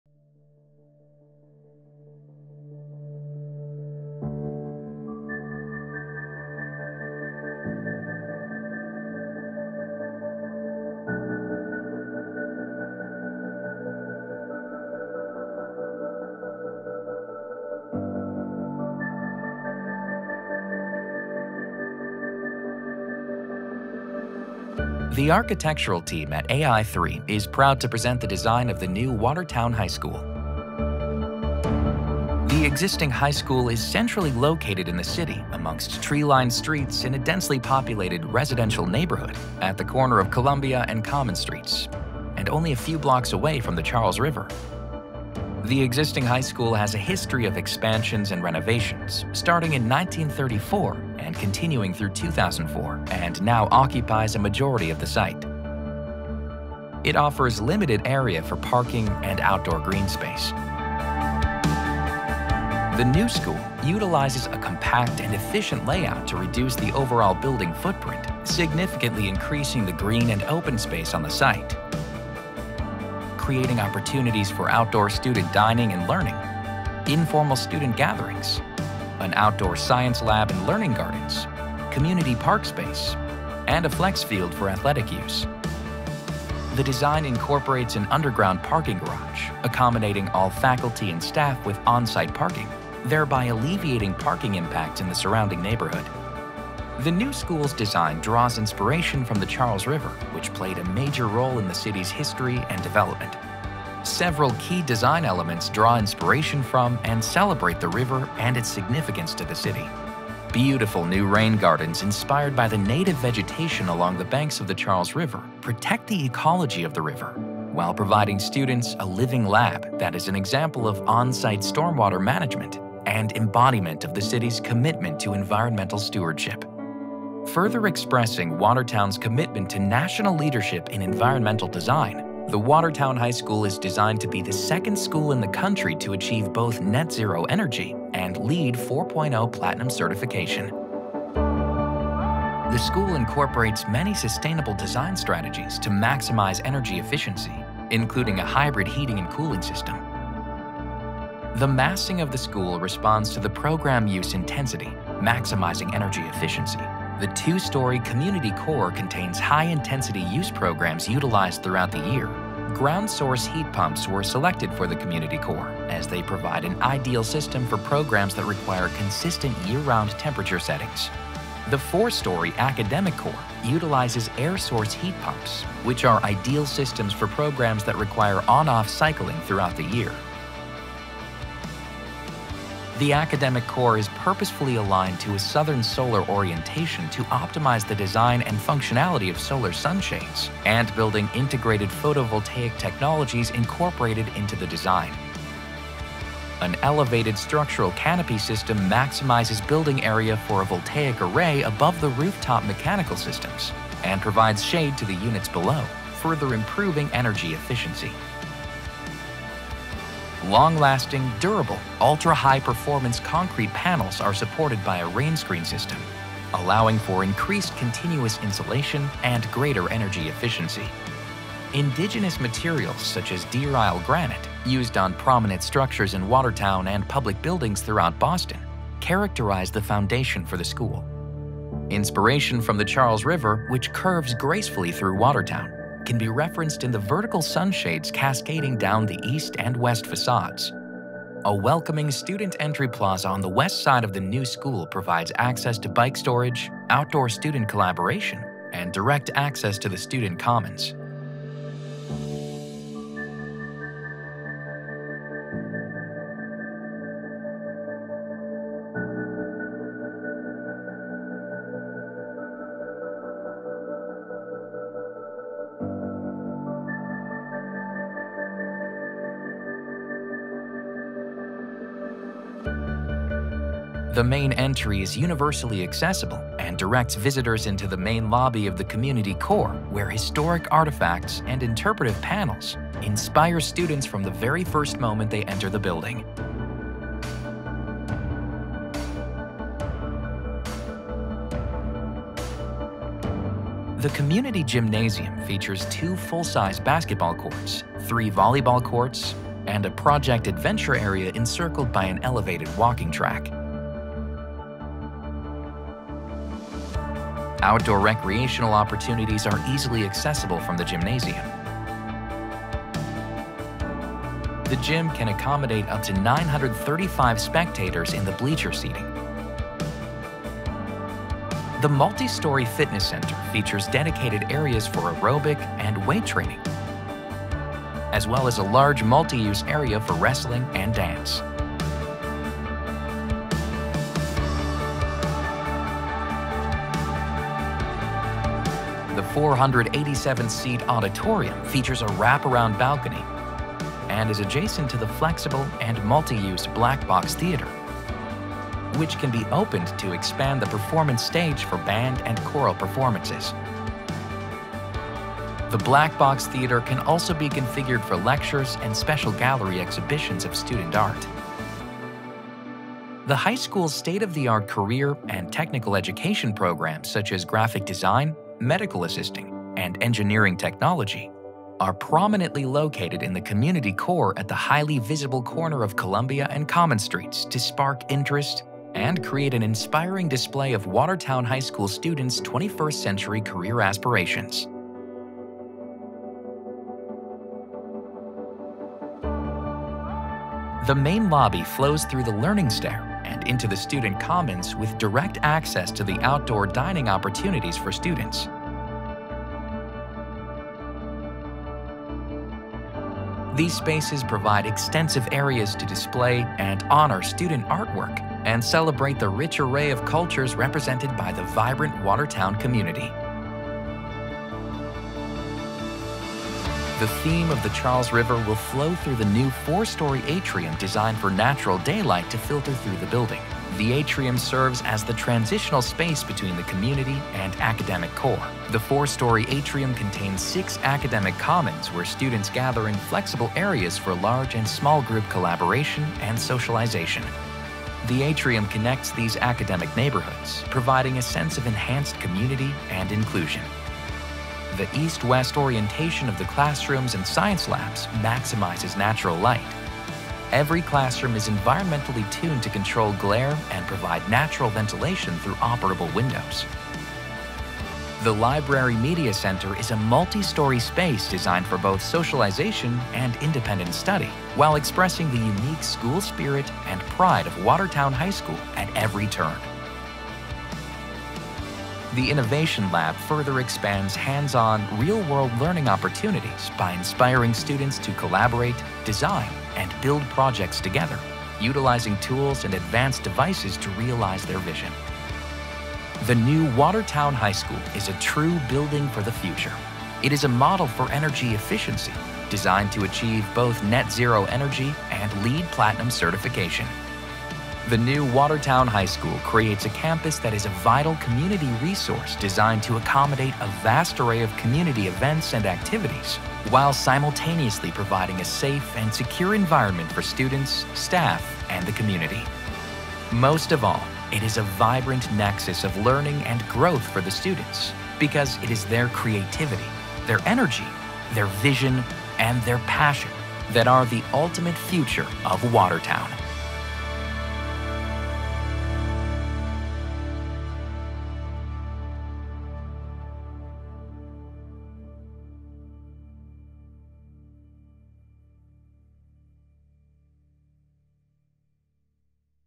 Watertown High School - Virtual Tour Architectural Animation
Animation of the future Watertown High School
In Watertown Massachusetts

Project Architect:
Ai3 Architects

Modeled by: Alax Neslon and Grant Rane of XR3D Studios
2D by: Samuel Riley of XR3D Studios
Rendered and Animated by: Grant Rane of XR3D Studios
Products used: Rhino 3D, 3D Studio Max, VRay, Anima, Railclone, Forestpack, SynthEyes, Photoshop, Illustrator, After Effects, and Premiere

Drone Footage by:
Ryan Fullam
Owner/Operator
HelioBook Photo & Video

Voice Over:
Greg Johnson